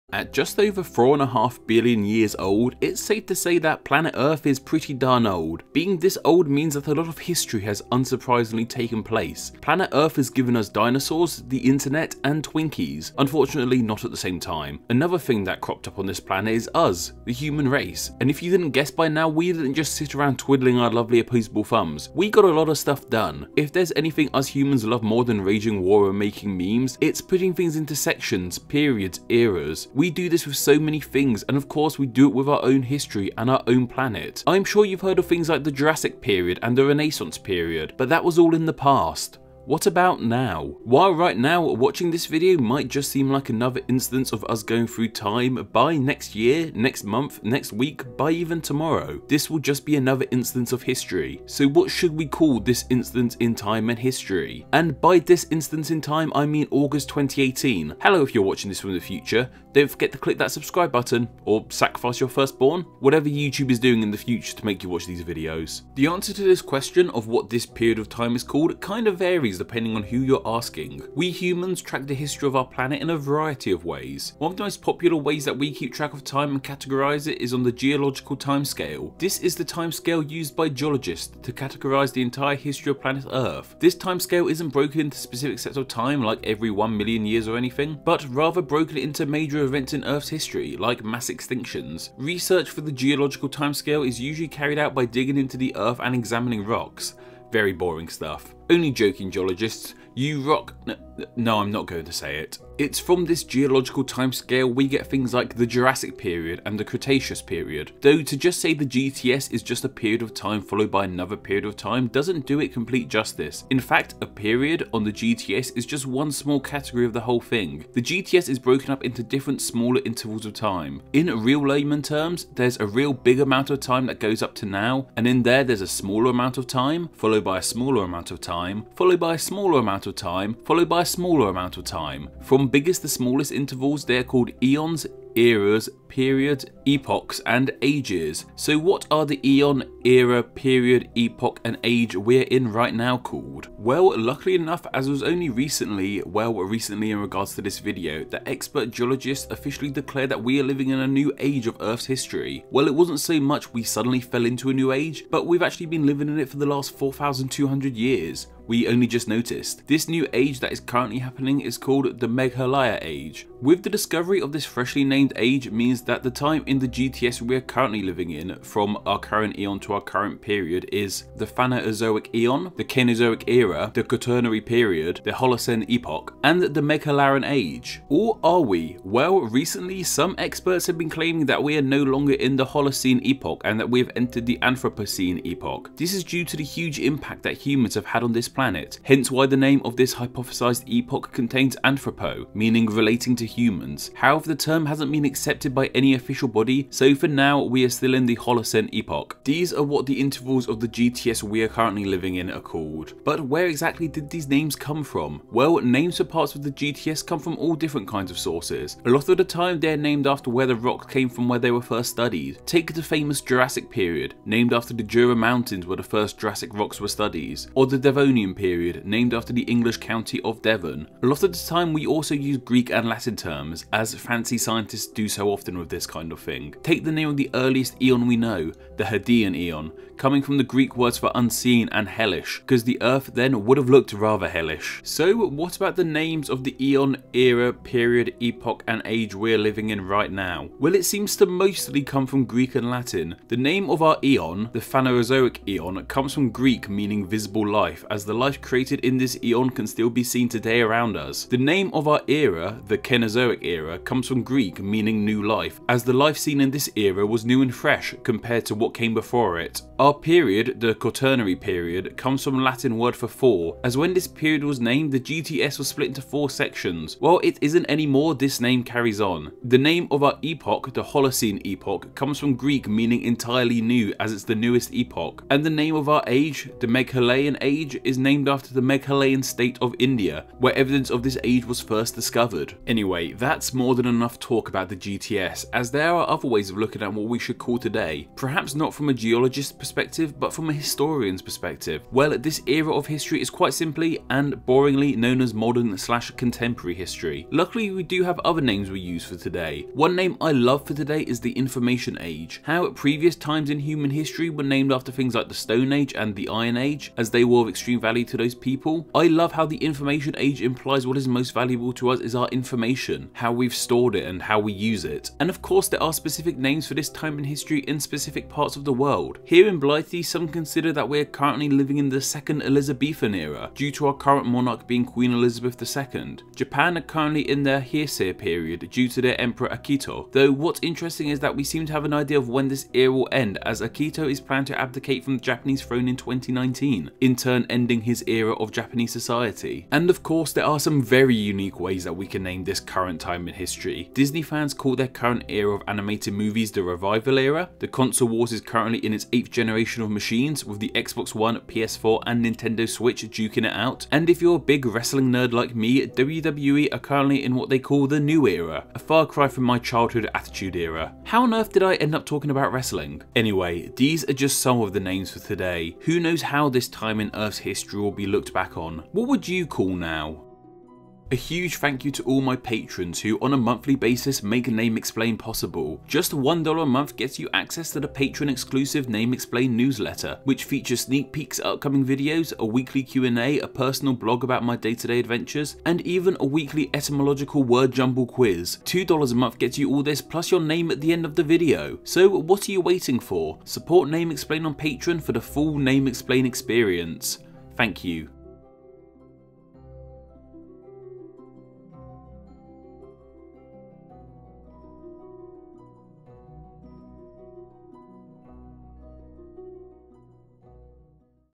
What Is This Time In History Called?
We Are Now Living In A New Geologic Age, Experts Say